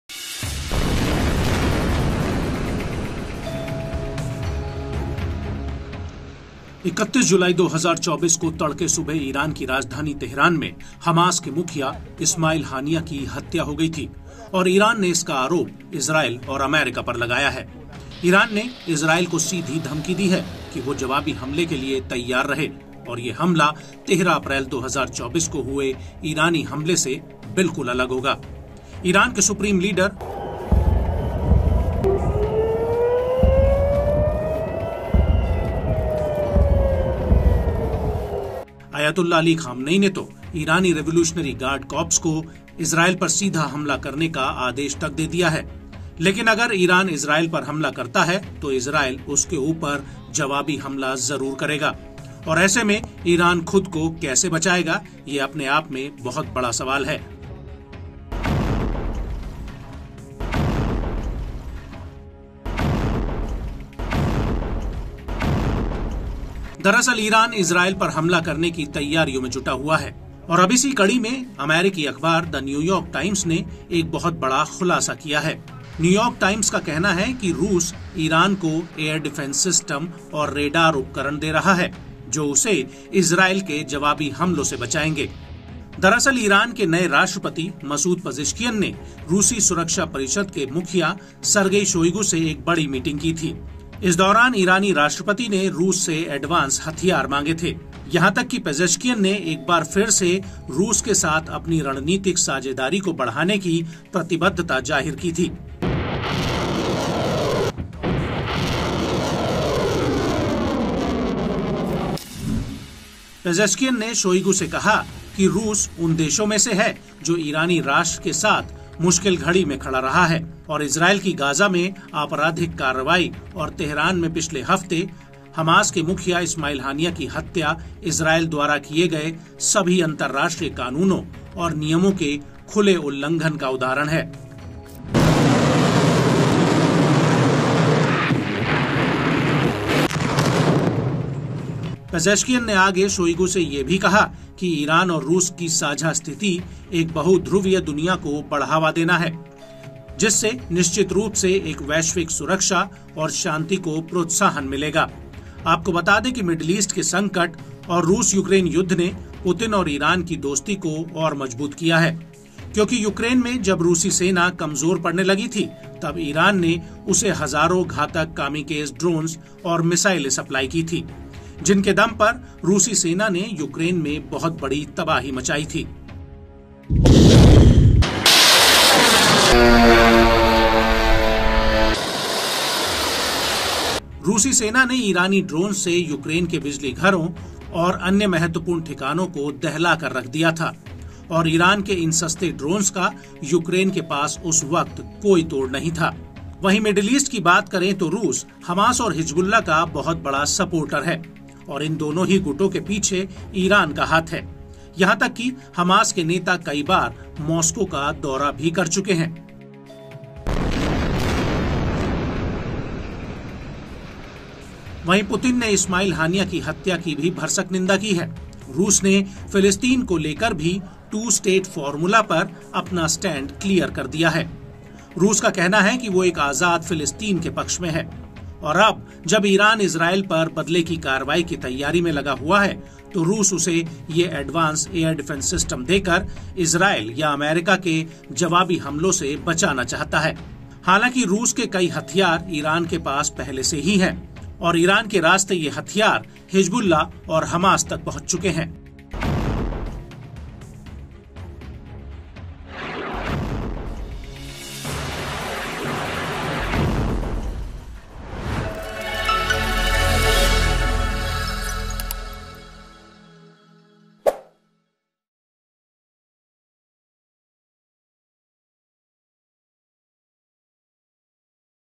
Israel Hamas War: Putin ने Iran को दिए Advance Air Defense System?| N18G|Gaza|Netanyahu
Hamas Leader Ismail Haniyeh died in Iran Capital Tehran on 31 July 2024 during the Israel Hamas War. Iran blamed Israel for this attack. Iran Supreme Leader Ayatollah Khamenei has even ordered IRGC to directly attack Israel. Now The New York Times says that Russia is giving Iran Advance Air Defense System and Radar Equipment, which will protect it from Israel Attack. In fact, Iran President Masoud Pezeshkian had a big meeting with Russian Security Council Chief Sergei Shoigu. During this, Iran President had asked for Advance Weapons from Russia. Even Pezeshkian once again expressed his commitment to increase his strategic partnership with Russia.

Israel Hamas War के बीच 31 जुलाई 2024 को Hamas Leader Ismail Haniyeh की मौत Iran Capital Tehran में हुई थी. Iran ने इस हमले का आरोप Israel पर लगाया था. Iran Supreme Leader Ayatollah Khamenei ने IRGC को Israel पर सीधे हमला का आदेश तक दे दिया है. अब The New York Times का कहना है कि Russia Iran ईरान को Advance Air Defense System और Radar Equipment दे रहा है, जो उसे Israel Attack से बचाएंगे. दरअसल, Iran President Masoud Pezeshkian ने Russian Security Council Chief Sergei Shoigu से एक बड़ी मीटिंग की थी. इस दौरान Iran President ने Russia से Advance Weapons मांगे थे. यहां तक कि पेजेश्कियन ने एक बार फिर से रूस के साथ अपनी रणनीतिक साझेदारी को बढ़ाने की प्रतिबद्धत्ता ज़ाहिर की थी.